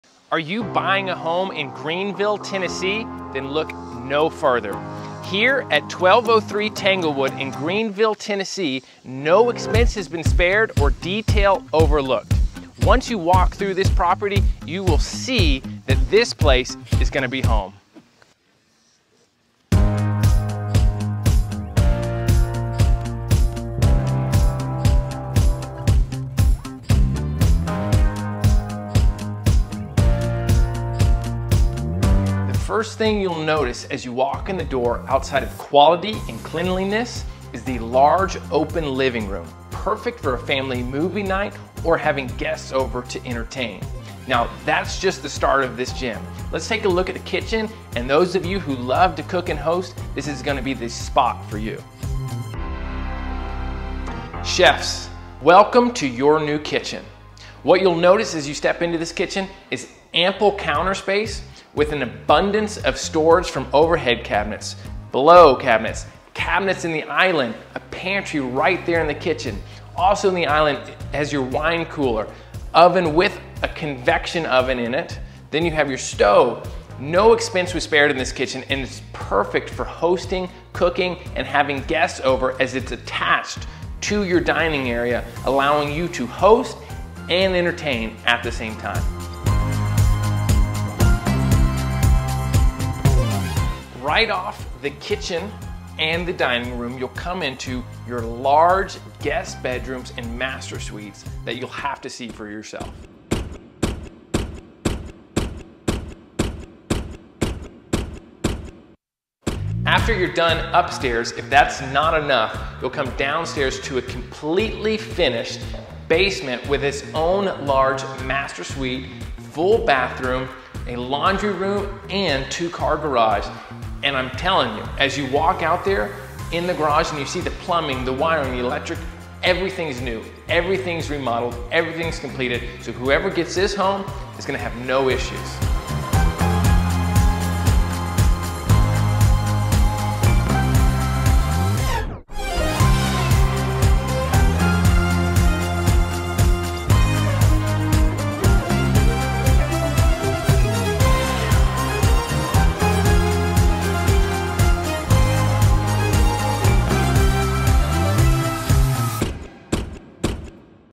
Exclusive Tour: Move-In Ready Gem at 1203 Tanglewood Dr, Greenville TN!
Schedule a Buyer or Seller Consultation with The Honeycomb Group

Discover this impeccably remodeled 3-bedroom, 3-bathroom ranch-style home, where no detail has been overlooked. Designed as a forever home, this property boasts a spacious, open living room perfect for gatherings, and a chef's kitchen with abundant counter space and cabinetry that flows seamlessly into the dining area. Off the dining space, a sunlit room offers the ideal setting for an office or intimate conversations.

The fully finished basement is a standout feature, hosting a luxurious master suite, full bathroom, cozy living area, and a convenient laundry room. Every corner of this home reflects meticulous attention to detail, with updates including electrical, plumbing, HVAC, new roof, driveway, retaining wall, fencing, and so much more. This home is the epitome of quality craftsmanship and thoughtful design, ready for you to move in and call home.